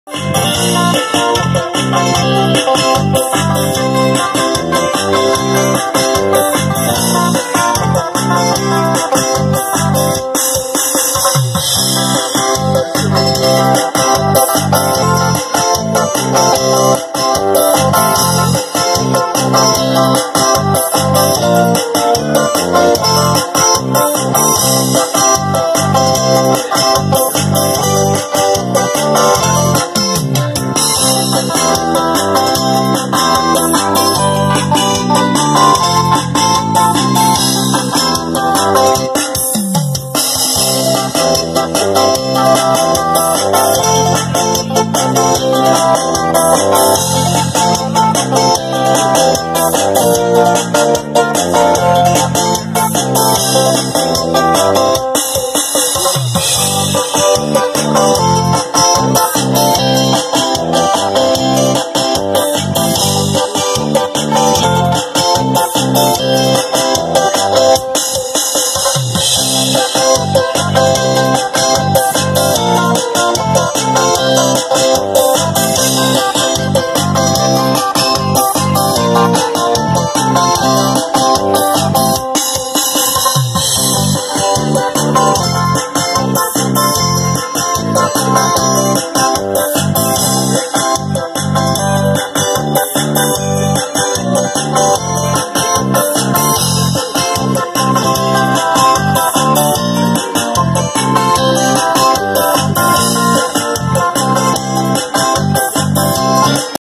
Edson Pereira e o cantor piranha, tocando um bregão romântico para os corações apaixonados 😀
Edson Pereira e o cantor piranha tocando um bregão romântico para os corações apaixonados pelo Brasil e o mundo inteiro 👏